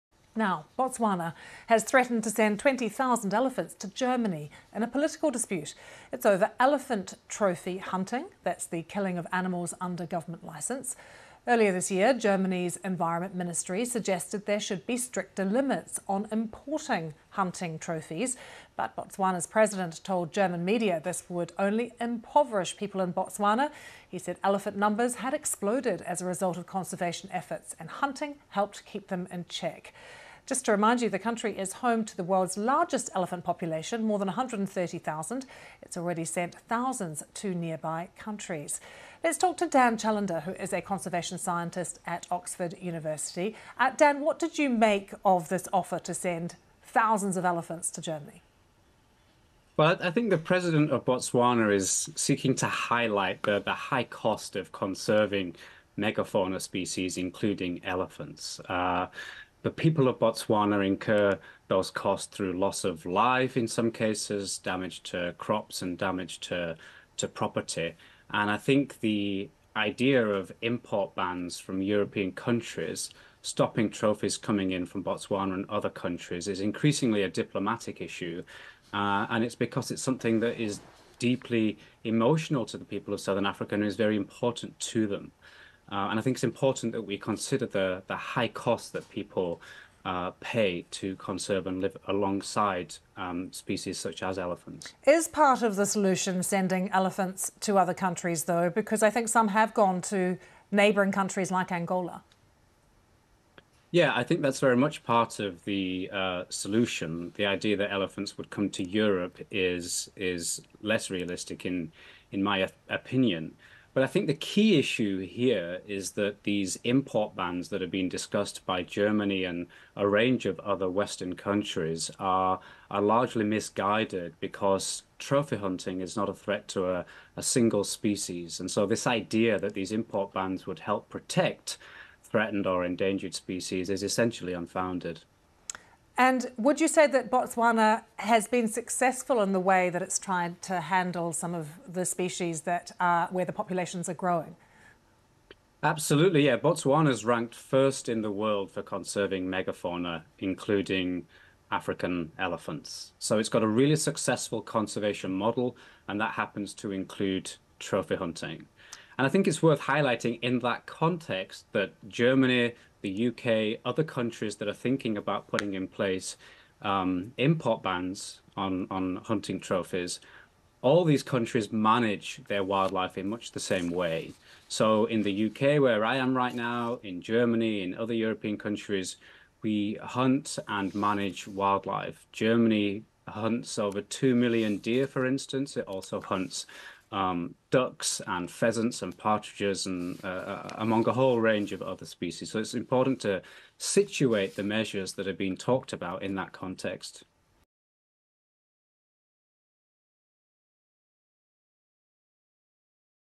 Botswana threatens to send 20,000 elephants to Germany-
بوٹسوانا نے 20,000 ہاتھی جرمنی بھیجنے کی دھمکی دی ہے۔

Botswana droht, 20.000 Elefanten nach Deutschland zu schicken

Le Botswana menace d'envoyer 20 000 éléphants en Allemagne-

بوتسوانا تهدد بإرسال 20 ألف فيل إلى ألمانيا-

The president of Botswana has threatened to send 20,000 elephants to Germany in a dispute over conservation. Earlier this year, Germany's environment ministry suggested there should be stricter limits on importing trophies from hunting animals. Botswana's President Mokgweetsi Masisi told German media this would only make people poorer in his country. He said elephant numbers had exploded as a result of conservation efforts, and hunting helped keep them in check.

بوٹسوانا کے صدر نے تحفظ کے تنازع میں 20 ہزار ہاتھیوں کو جرمنی بھیجنے کی دھمکی دی ہے۔ اس سال کے شروع میں، جرمنی کی وزارت ماحولیات نے تجویز پیش کی تھی کہ شکار کرنے والے جانوروں سے ٹرافیاں درآمد کرنے پر سخت پابندیاں ہونی چاہئیں۔ بوٹسوانا کے صدر Mokgweetsi Masisi نے جرمن میڈیا کو بتایا کہ اس سے ان کے ملک میں لوگ غریب تر ہوں گے۔ انہوں نے کہا کہ ہاتھیوں کی تعداد تحفظ کی کوششوں کے نتیجے میں پھٹ گئی تھی، اور شکار نے انہیں قابو میں رکھنے میں مدد ک

Der Präsident von Botswana hat im Streit um den Artenschutz damit gedroht, 20.000 Elefanten nach Deutschland zu schicken. Anfang des Jahres schlug das deutsche Umweltministerium vor, strengere Beschränkungen für den Import von Trophäen von Jagdtieren einzuführen. Botswanas Präsident Mokgweetsi Masisi sagte gegenüber deutschen Medien, dass dies die Menschen in seinem Land nur ärmer machen würde. Er sagte, die Zahl der Elefanten sei aufgrund der Schutzbemühungen explodiert und die Jagd habe dazu beigetragen, sie unter Kontrolle zu halten.

Le président du Botswana a menacé d'envoyer 20 000 éléphants en Allemagne dans le cadre d'un différend sur la conservation. Plus tôt cette année, le ministère allemand de l'Environnement a suggéré qu'il devrait y avoir des limites plus strictes à l'importation de trophées d'animaux de chasse. Le président du Botswana, Mokgweetsi Masisi, a déclaré aux médias allemands que cela ne ferait qu'appauvrir la population de son pays. Il a déclaré que le nombre d'éléphants avait explosé grâce aux efforts de conservation et que la chasse avait contribué à les contrôler.

هدد رئيس بوتسوانا بإرسال 20 ألف فيل إلى ألمانيا في نزاع حول الحفاظ على الفيلة. وفي وقت سابق من هذا العام، اقترحت وزارة البيئة الألمانية ضرورة فرض قيود أكثر صرامة على استيراد الجوائز من حيوانات الصيد. وقال رئيس بوتسوانا موكجويتسي ماسيسي لوسائل الإعلام الألمانية إن هذا لن يؤدي إلا إلى زيادة فقر الناس في بلاده. وأضاف أن أعداد الأفيال تزايدت نتيجة لجهود الحفاظ على البيئة، وأن الصيد ساعد في السيطرة عليها.